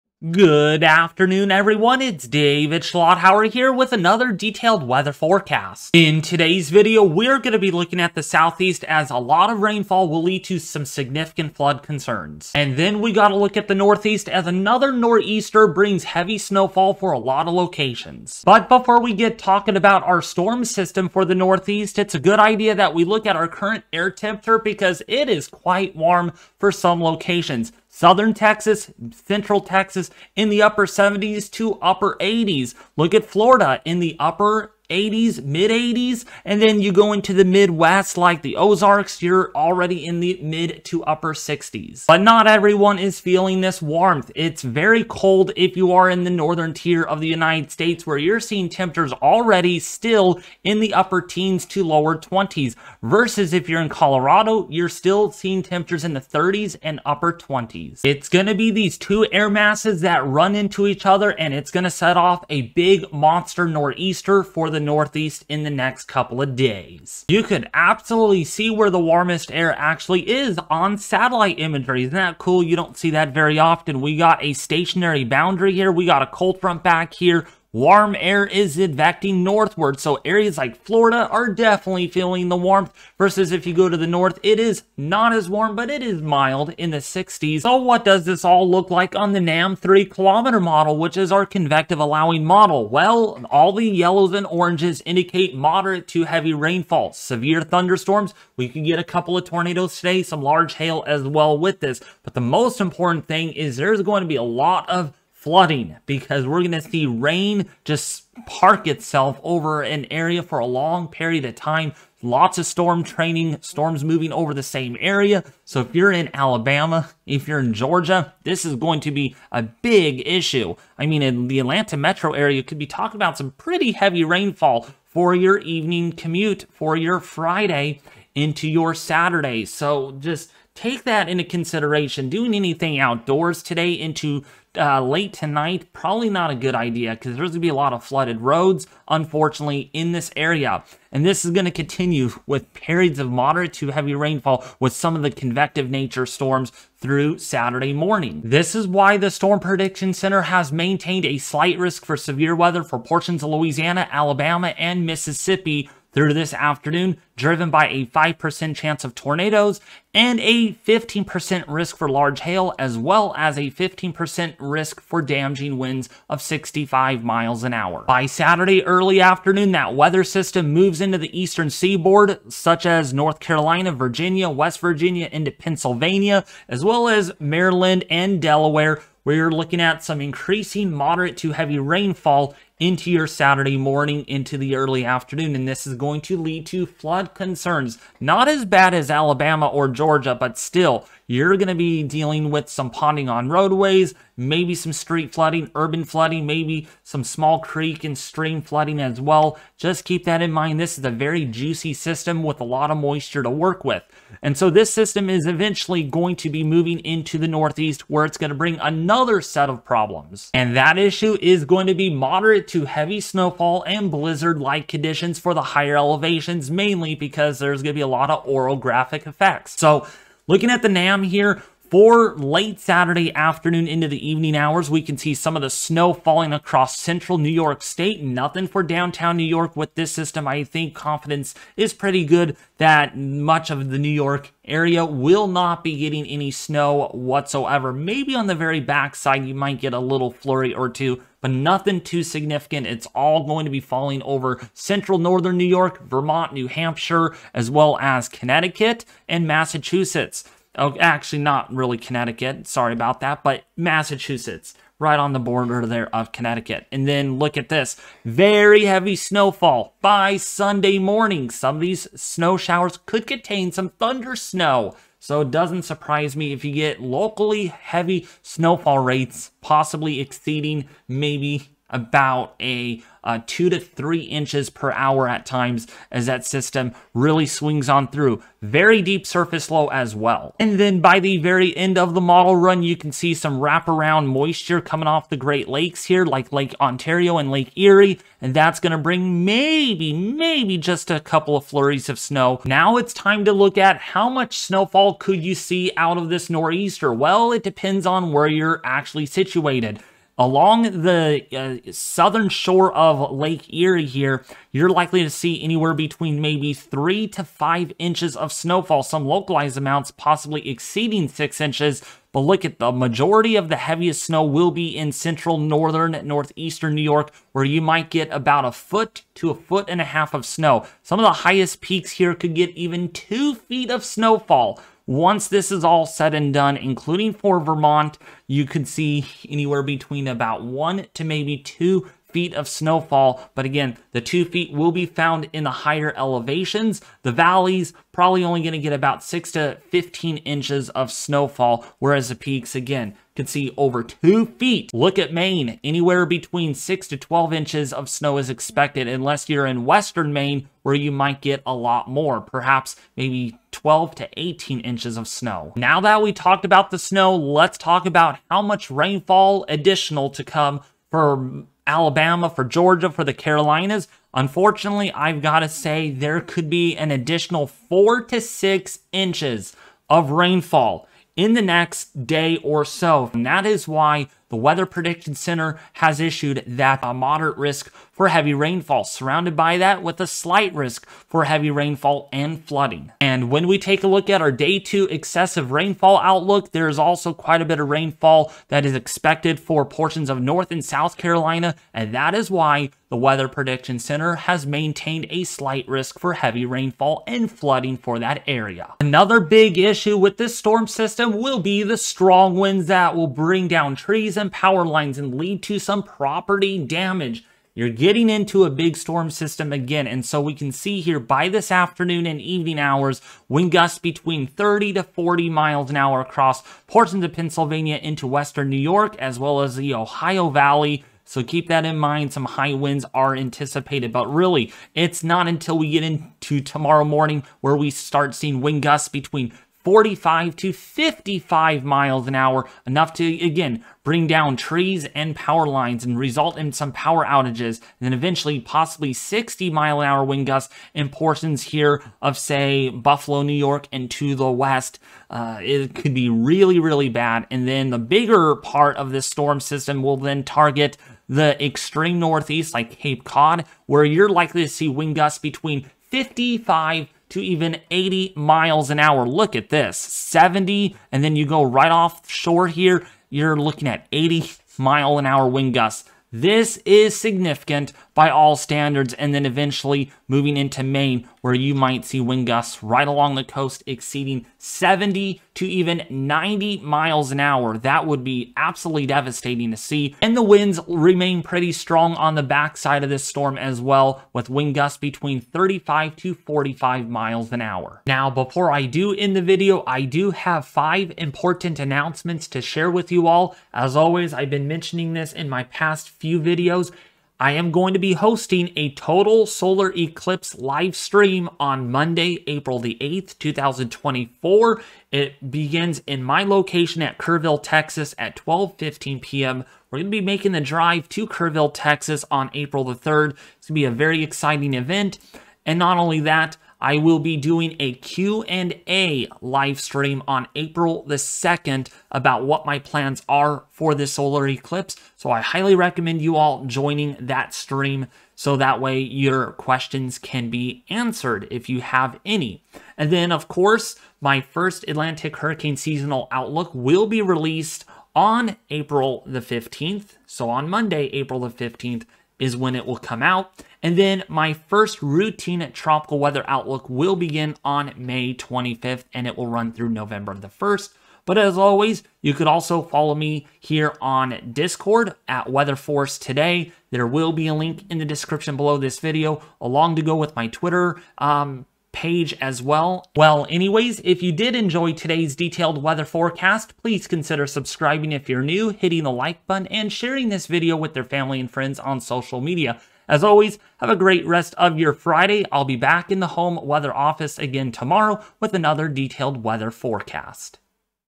This Storm Will Grow Much Bigger...
A storm system in the deep south is about to explode as it bombs out into a powerful nor'easter. Latest models continue to indicate several inches to as much as 2 feet of snow across the northeast, but what about the severe weather threat & flood threat?

Video Chapters:
0:00 - Intro
0:17 - Weather Right Now
1:27 - Big Rains & Flood Concerns
3:45 - A Big Nor'easter Is Coming
4:52 - Big Snow Totals
5:55 - WPC Products
6:40 - High Winds Expected
8:08 - Important Announcements
9:26 - Outro/Promotion